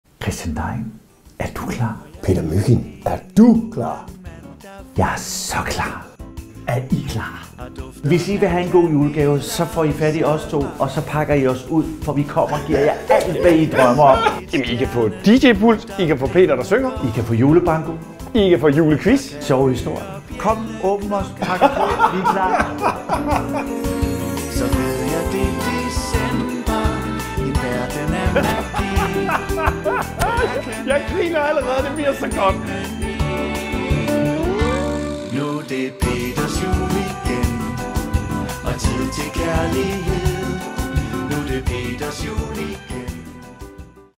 Juleshow med PETER MYGIND og CHRISTIAN DEGN
Vi er klar til at levere sublim underholdning til årets julefrokoster! Mygind og Degn underholder med julebanko/julequiz, masser af musik og sang - og ikke mindst - den bedste juleenergi.